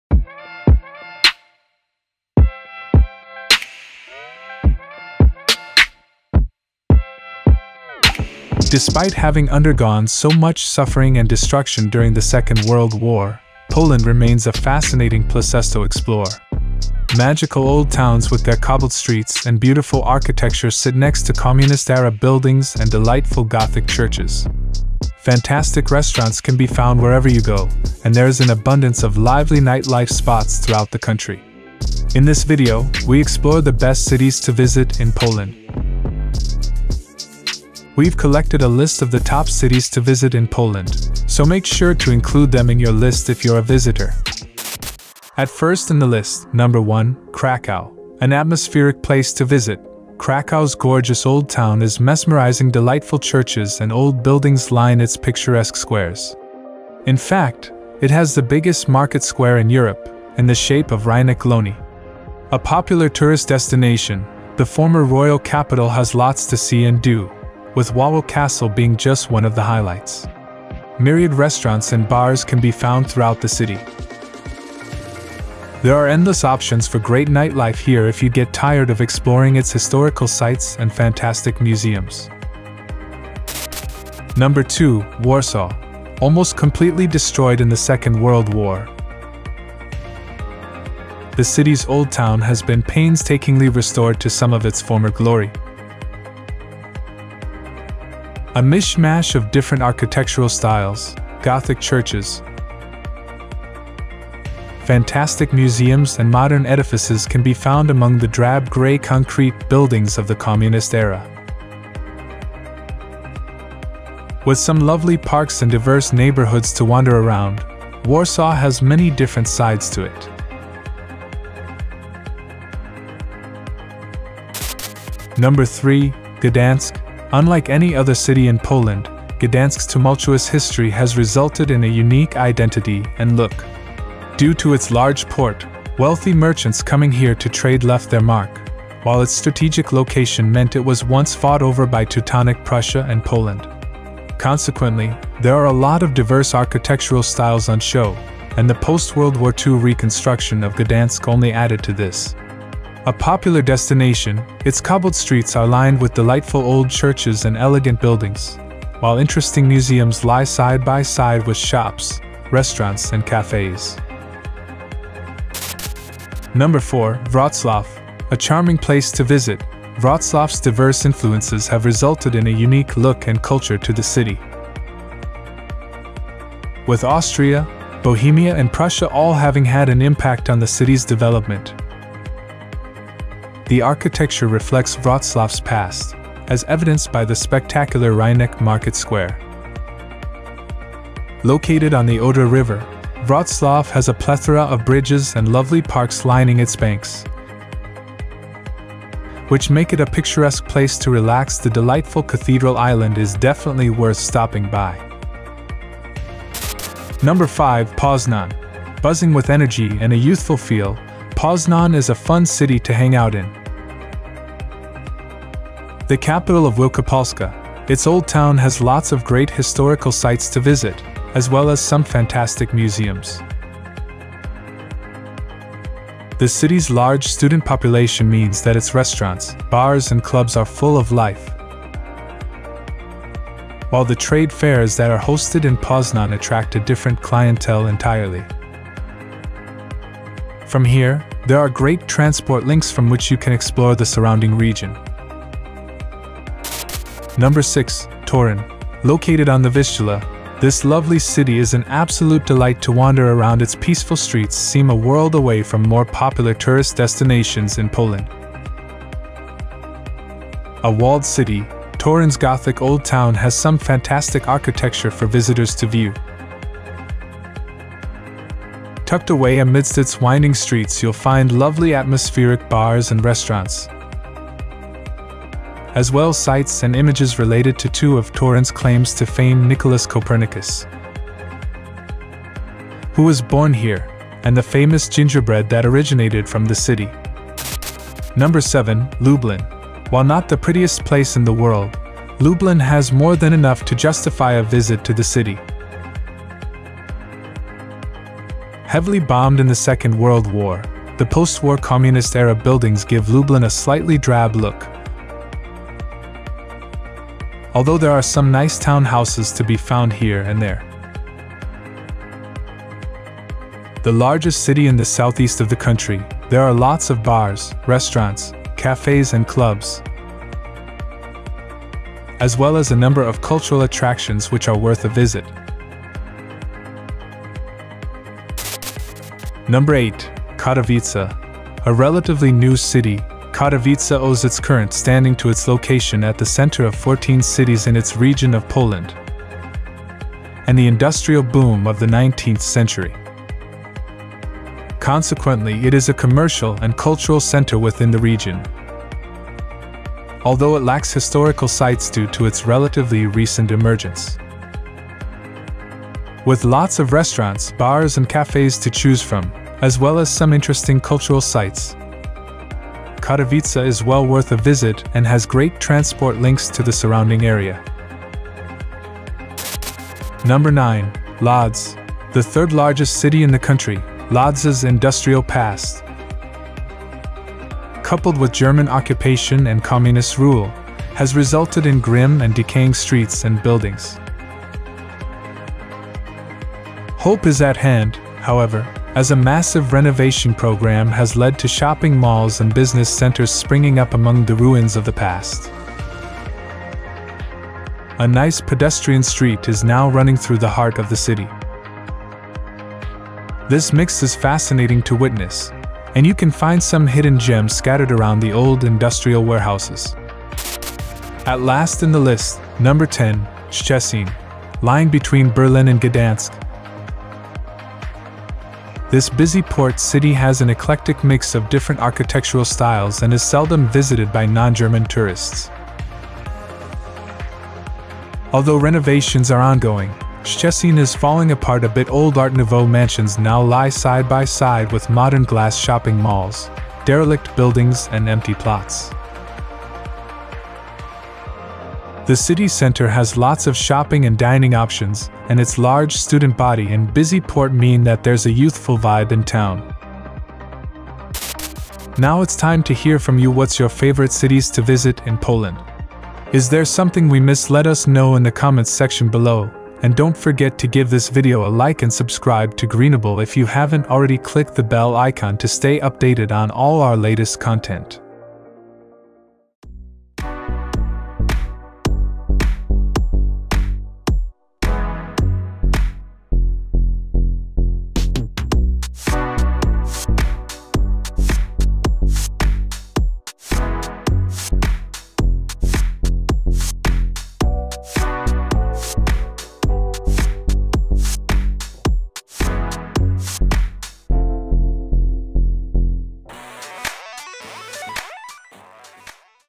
Top 10 Best Cities to Visit in Poland - Travel Video 2024
Are you planning a trip to Poland and wondering which cities to visit? Look no further! In this video, we will explore the best cities in Poland that you must add to your itinerary.

Warsaw: The vibrant capital city of Poland is a perfect blend of tradition and modernity. Explore the historic Old Town, visit the Royal Castle, and immerse yourself in the rich cultural heritage of Warsaw.

Krakow: Known for its charming Old Town and stunning architecture, Krakow is a must-visit city in Poland. Don't miss the famous Wawel Castle, the historic Jewish Quarter, and the somber Auschwitz-Birkenau Memorial and Museum.

Gdansk: Located on the Baltic coast, Gdansk is a picturesque city with a rich maritime history. Explore the colorful facades of Long Market, visit the Museum of the Second World War, and relax on the sandy beaches of Sopot.

Wroclaw: With its beautiful market square, stunning Gothic architecture, and lively cultural scene, Wroclaw is a hidden gem in Poland. Don't miss the iconic Ostrow Tumski, the Centennial Hall, and the lively Rynek.

Poznan: This charming city in western Poland is known for its vibrant nightlife, historic architecture, and delicious cuisine. Explore the Old Market Square, visit the impressive Poznan Cathedral, and enjoy a stroll along the picturesque Malta Lake.

Whether you are a history buff, a foodie, or a nature lover, Poland has something to offer for everyone. So pack your bags and get ready to explore the best cities in this beautiful country.

Chapters:

00:00 Introduction
00:44 Krakow
01:23 Warsaw
02:04 Gdansk
02:45 Wroclaw
03:26 Poznan
04:07 Torun
04:48 Lublin
05:29 Katowice
06:10 Lodz
06:51 Szczecin